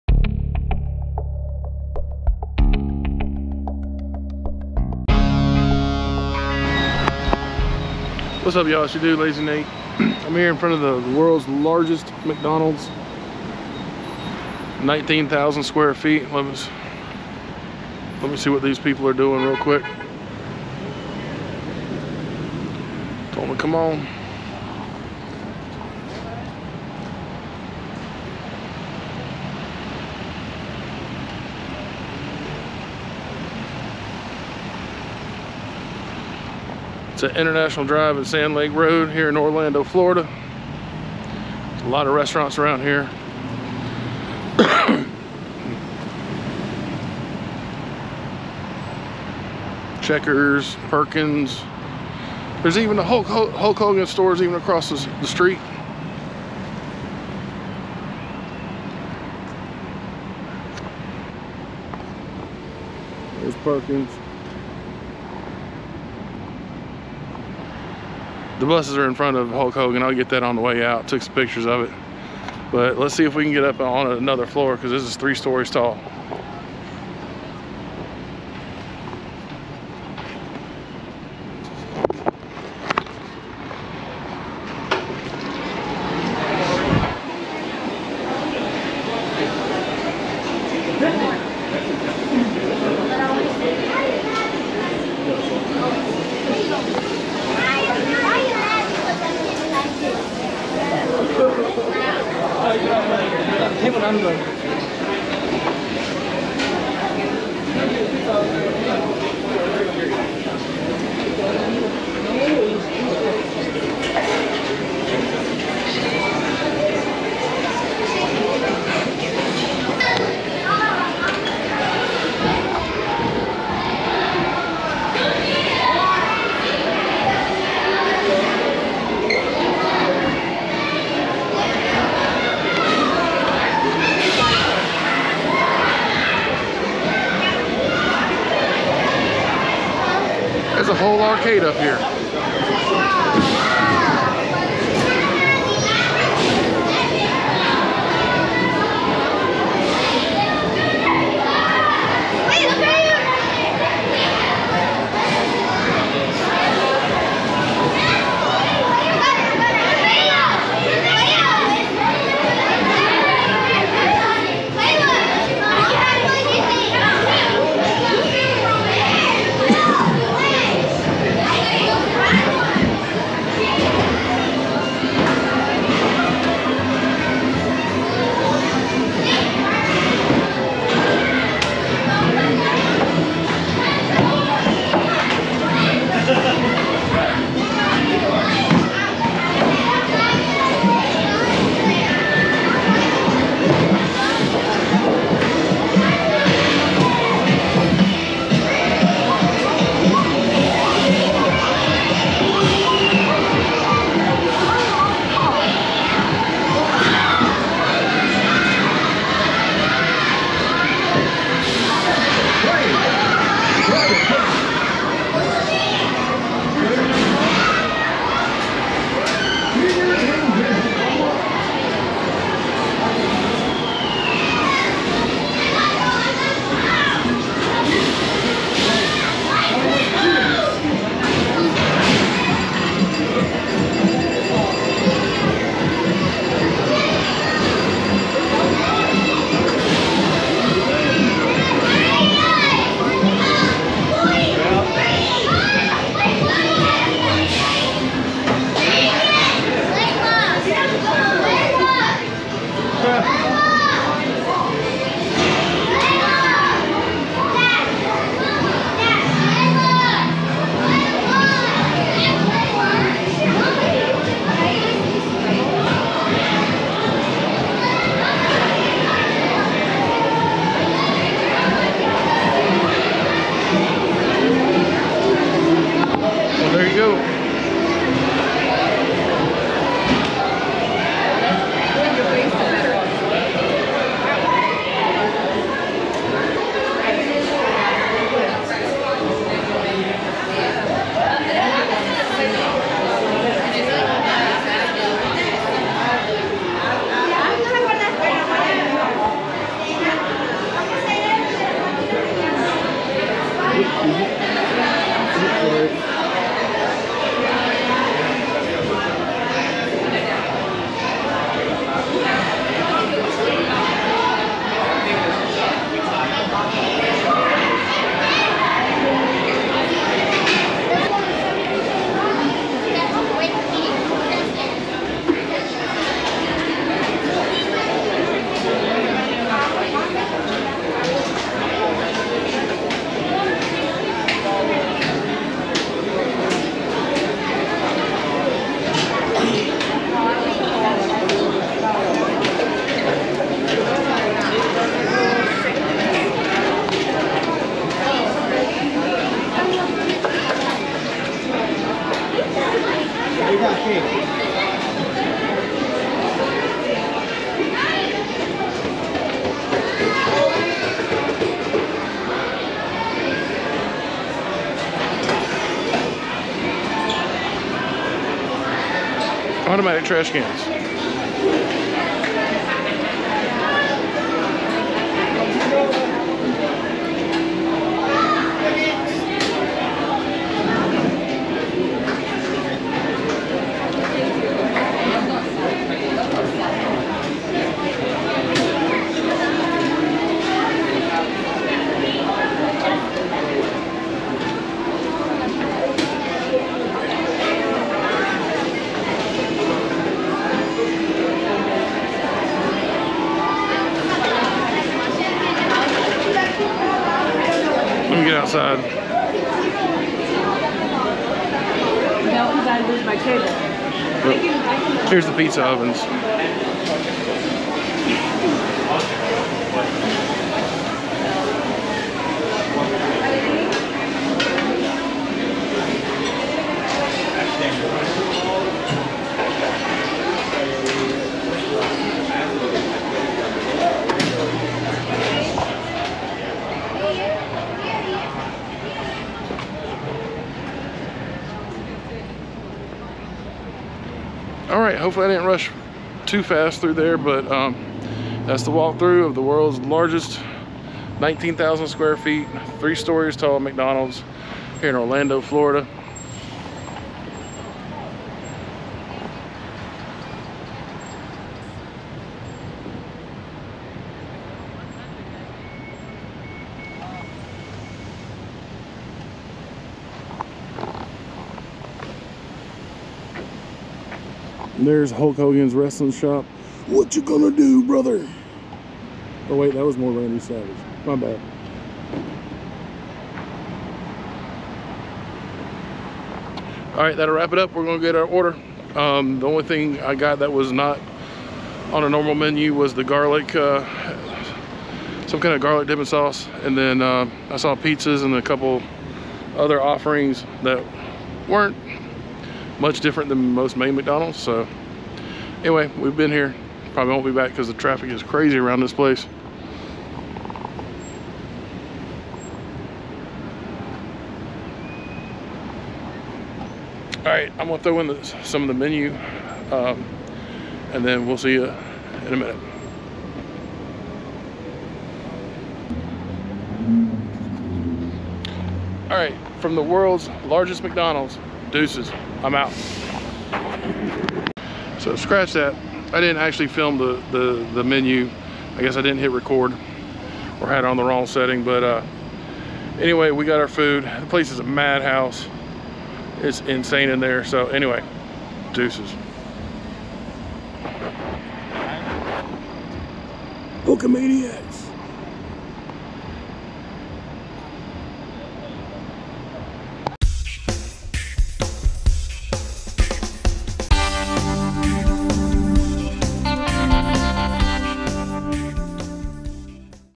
World's Largest McDonald's in Orlando, Fla
Take a look with me inside the world's largest McDonald's.
Clocking in at a whopping 19,000 sq ft. Pizza ovens, arcade, and more!